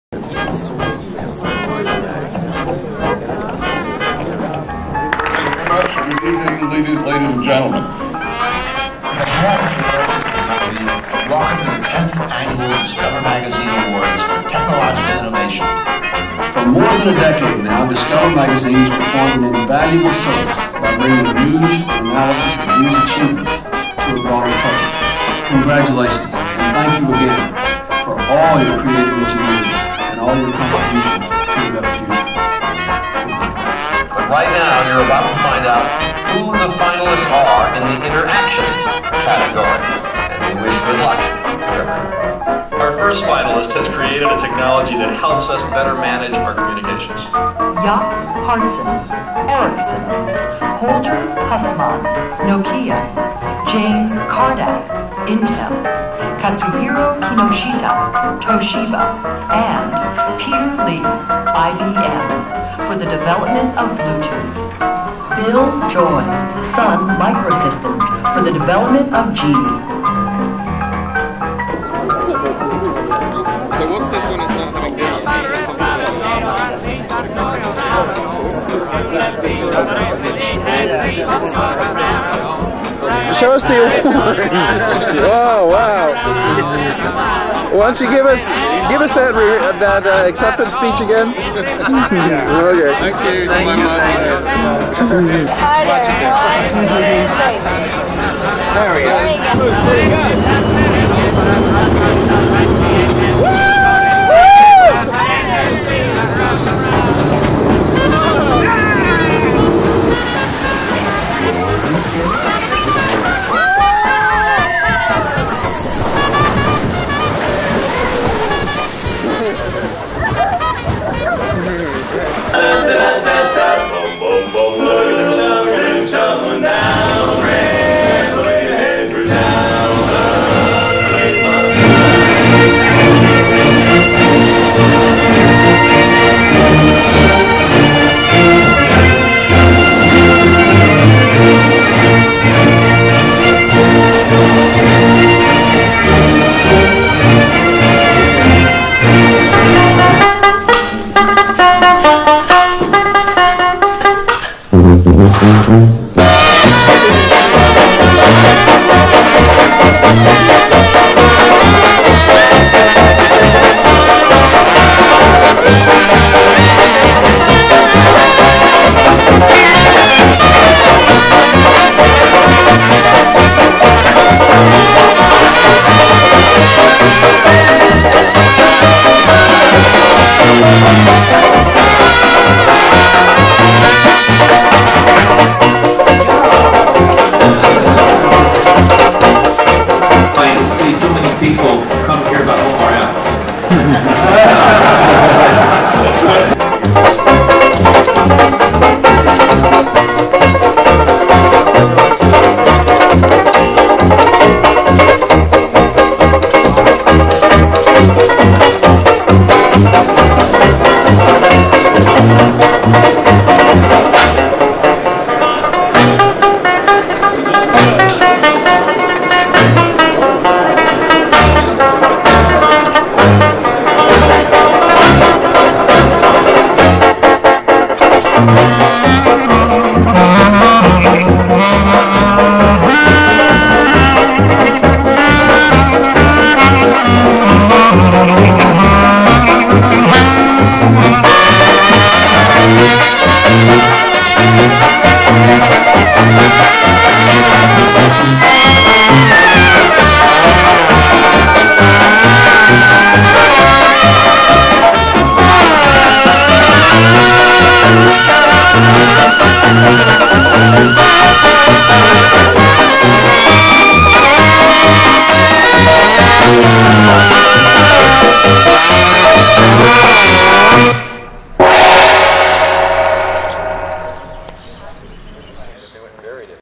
Bluetooth '99 Discover Awards & London Conf
In 1999 Bluetooth technology was a runner up for the Discover Magazine Award for Innovation (but lost to Sun's JINI).  This video shows a summary of the Discovery Awards ceremony at Disney World and the London Bluetooth conference that took place in August of 1999.